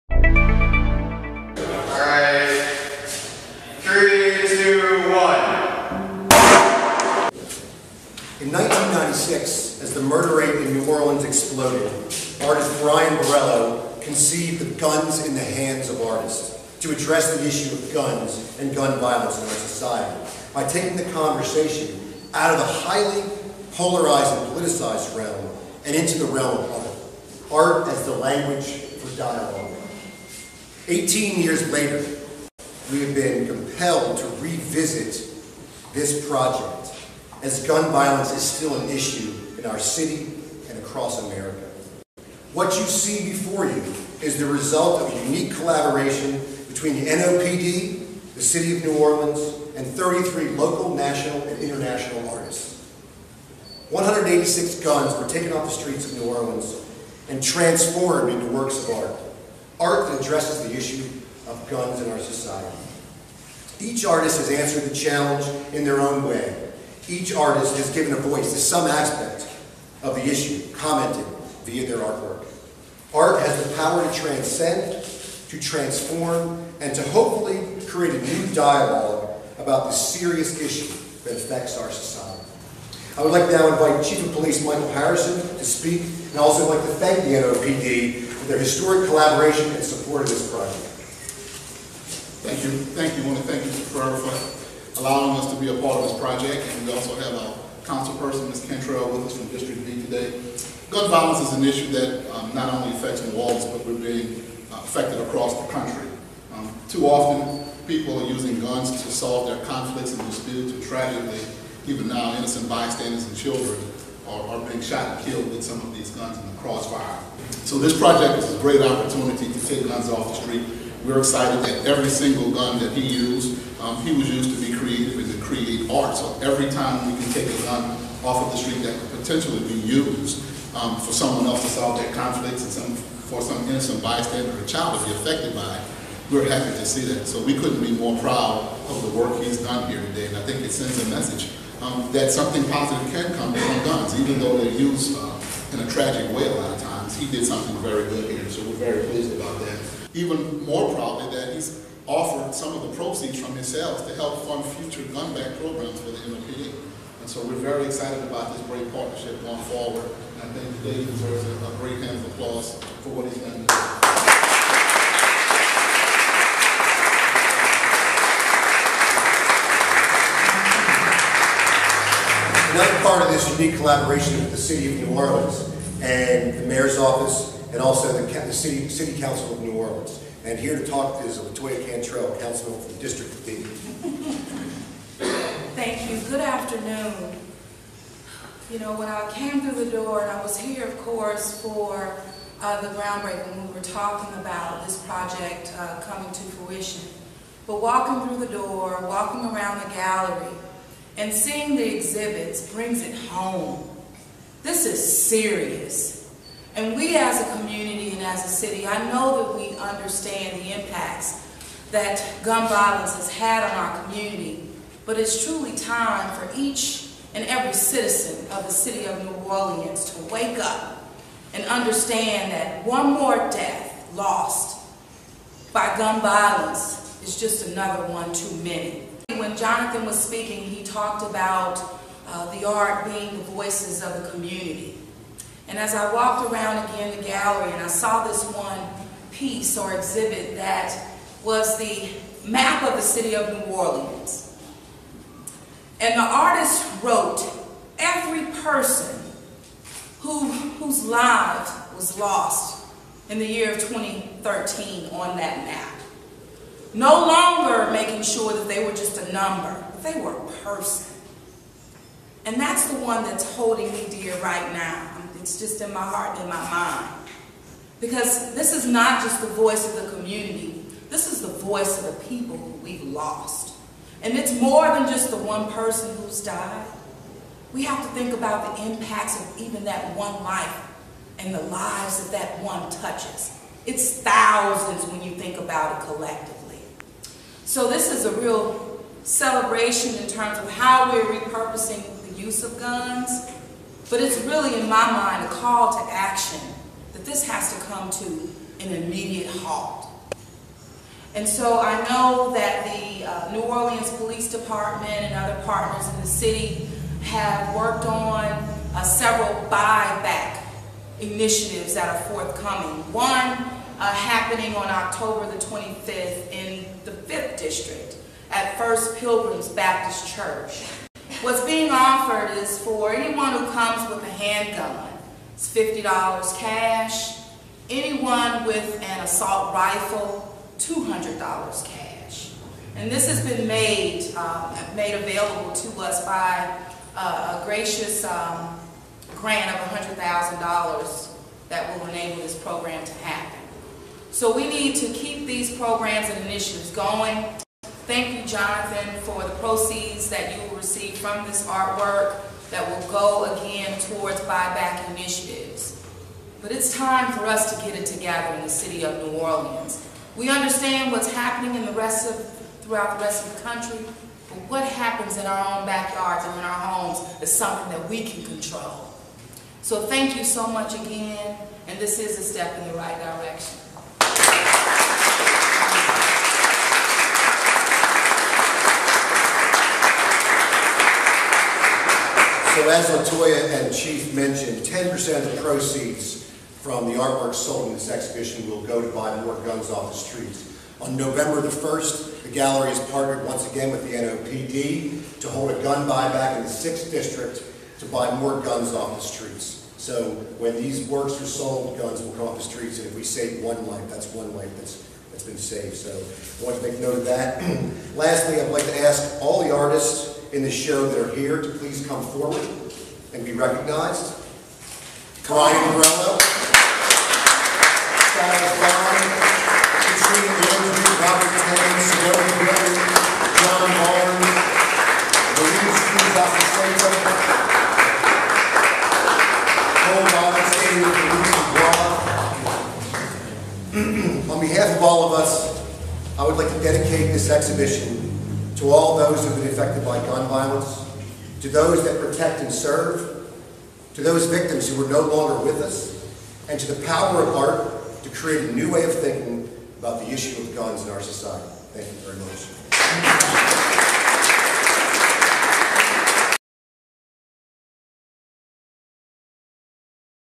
Guns in the Hands of Artists exhibit, in New Orleans
Watch as gallery owner Jonathan Ferrara, New Orleans Police Department Interim Superintendent Michael Harrison and New Orleans City Councilwoman LaToya Cantrell describe the conceptual art exhibit Guns in the Hands of Artists 2014.  In preparation